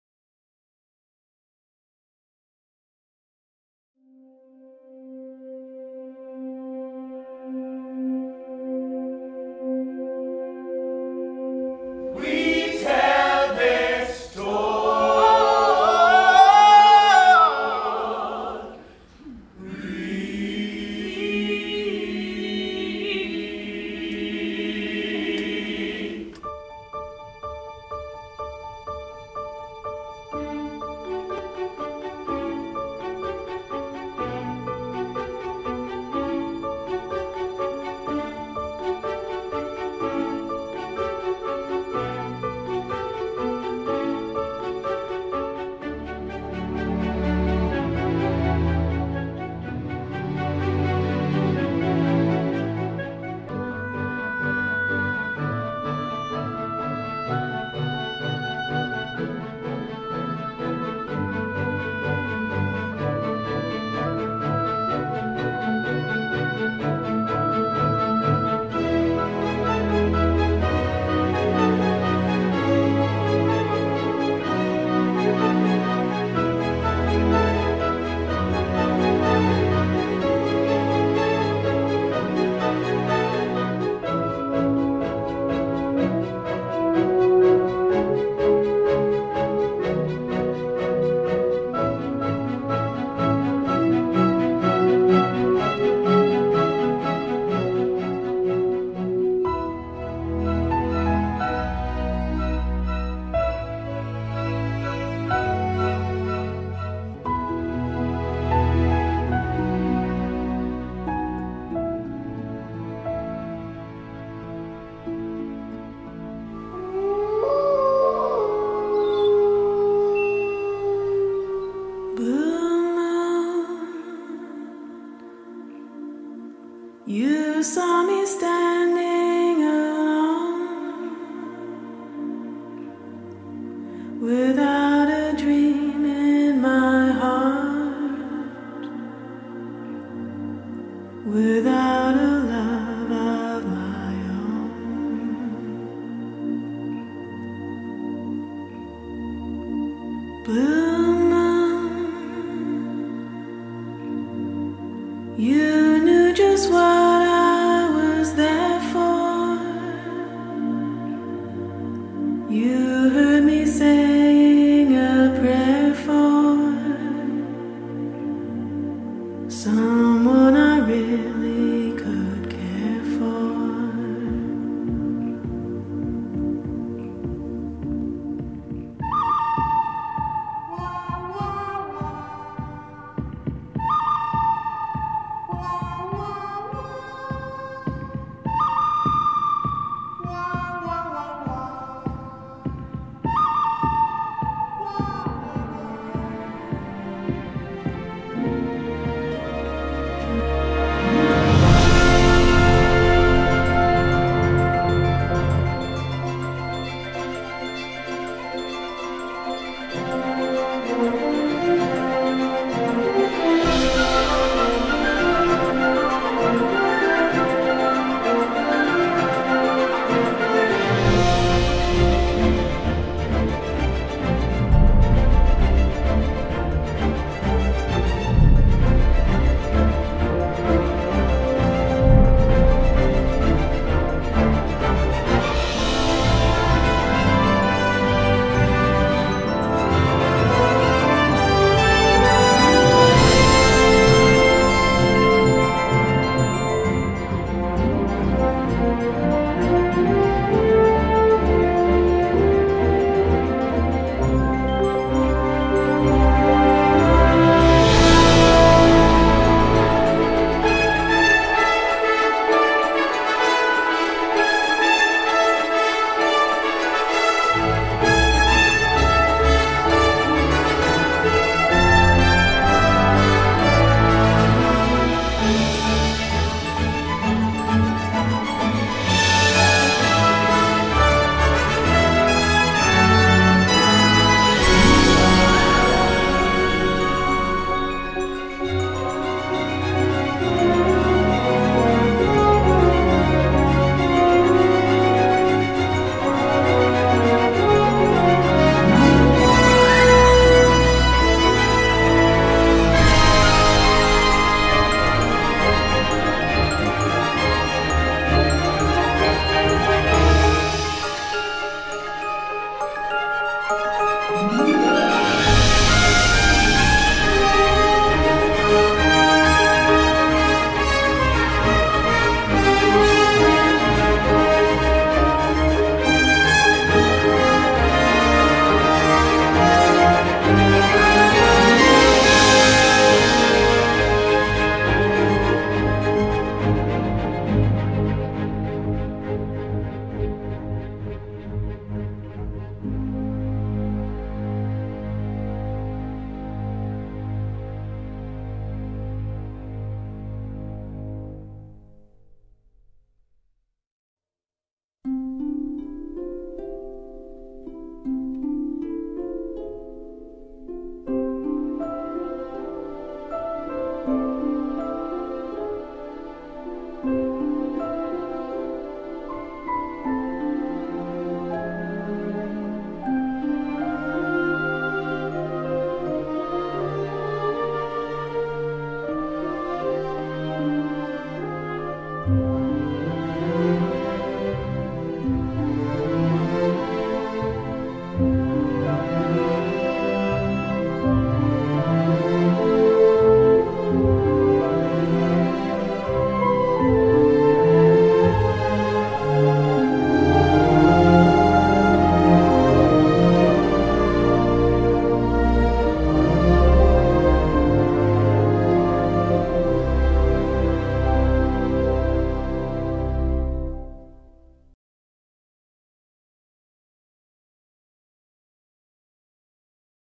Gainesville Theatre Alliance 2016
Images from the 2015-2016 Season of the Gainesville Theatre Alliance  - the nationally-acclaimed collaboration of the University of North Georgia, Brenau University, Guest Professionals and the Northeast Georgia Community